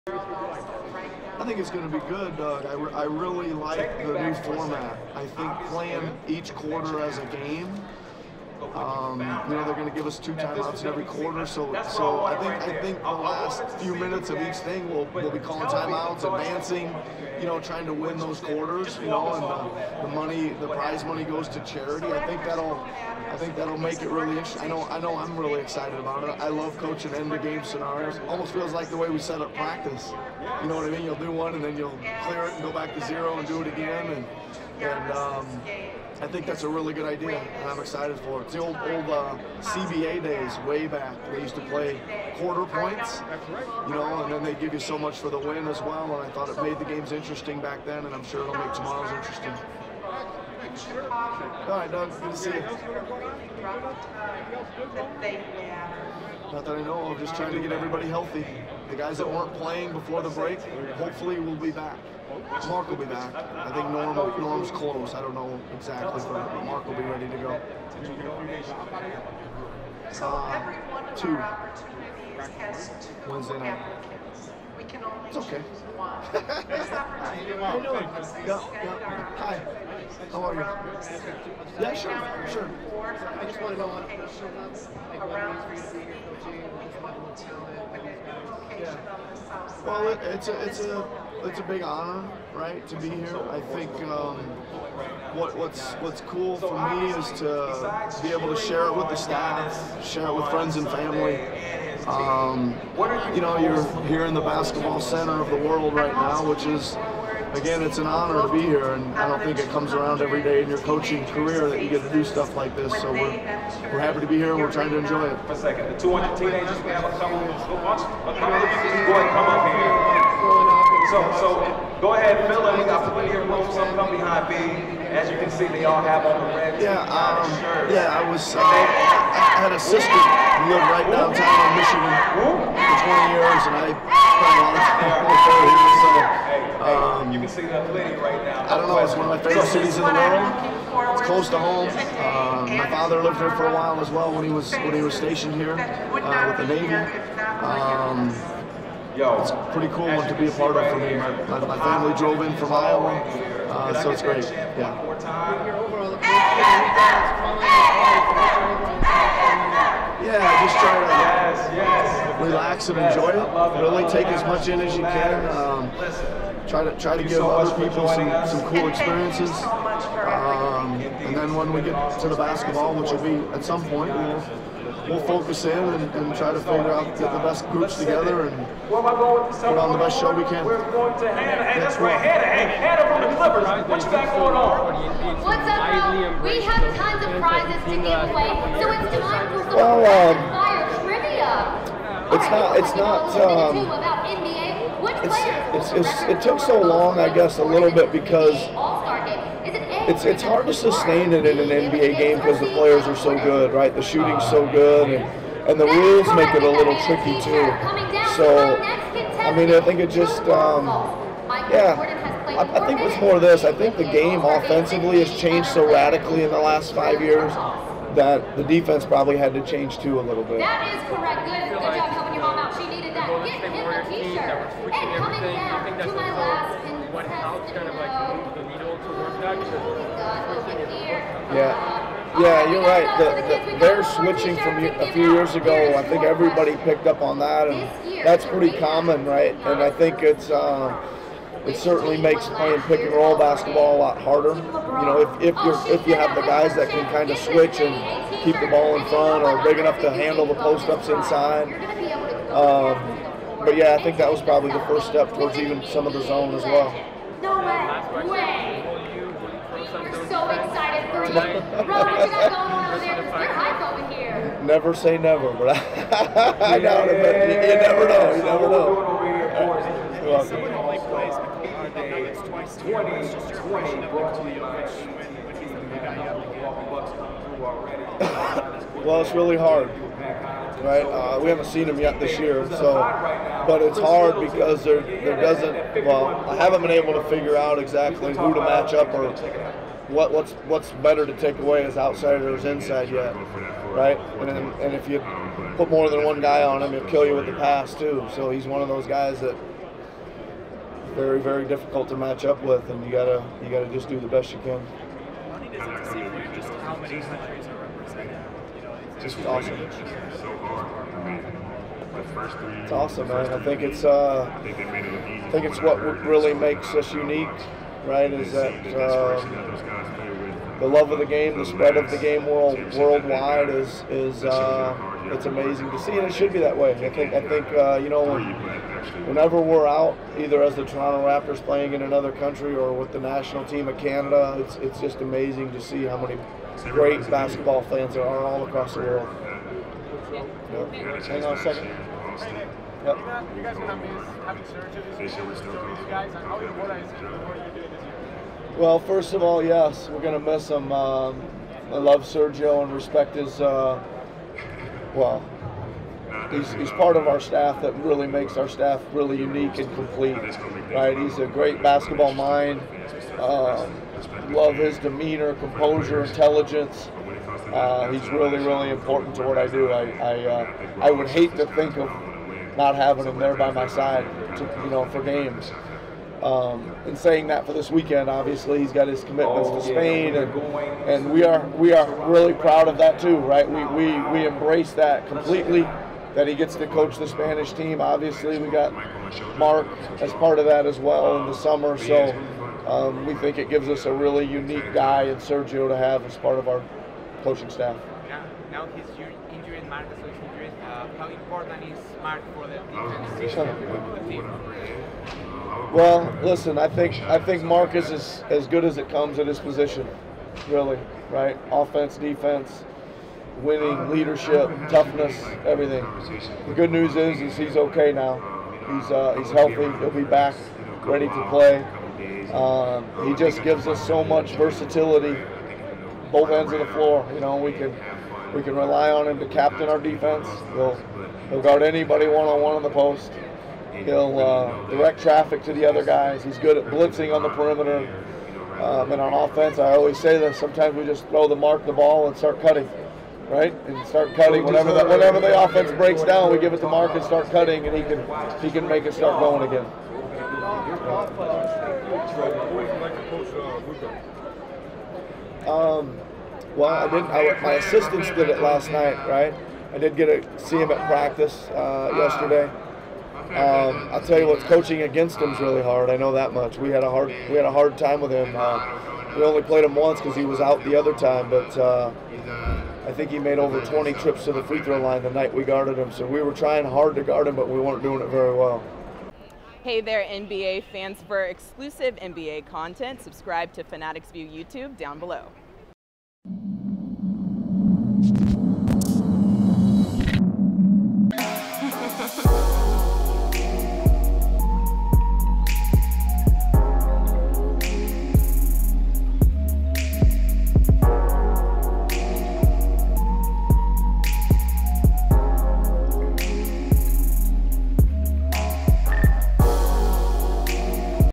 Nick Nurse on Raptors All-Star Experiences, Nikola Jokic & Marc Gasol | NBA ALL-STAR
Watch as Nick Nurse speaks on Raptors All-Star Experiences, Nikola Jokic & Marc Gasol